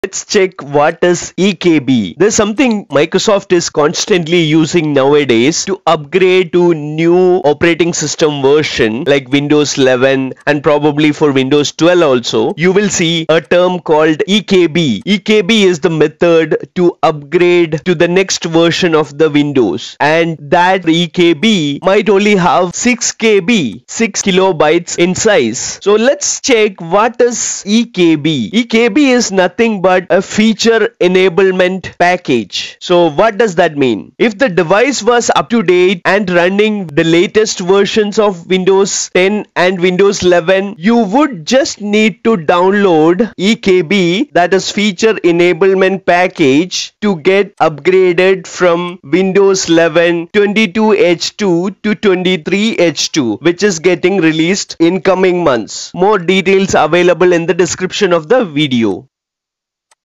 What is eKB?
What is eKB? How this eKB can help to upgrade the Windows 11 and Windows 12 versions on the fly. Let's also understand what is the average size of eKB updates? All these details are available in this video!

The enablement package is a great option for installing a scoped feature update like Windows 11, version 23H2, as it enables an update from version 22H2 with a single restart, reducing update downtime.

If the device was up to date and running Windows 11, version 22H2, you’d only download a 6KB enablement package! That means that you’d have the latest monthly update (LCU) installed, which can include the SSU. You’d only need to deploy the feature enablement package (eKB) for the
device to upgrade to Windows 11, version 23H2!

In the context of Microsoft, eKB can refer to an Enablement Package KB. An Enablement Package KB (EKB) is a package that makes updating to newer versions of Windows 11 faster and easier and requires only a single restart. To find the EKB for a specific OS, you can go to the Improvements section and click or tap the OS name to expand the collapsible section1.

It's the feature enablement package, called eKB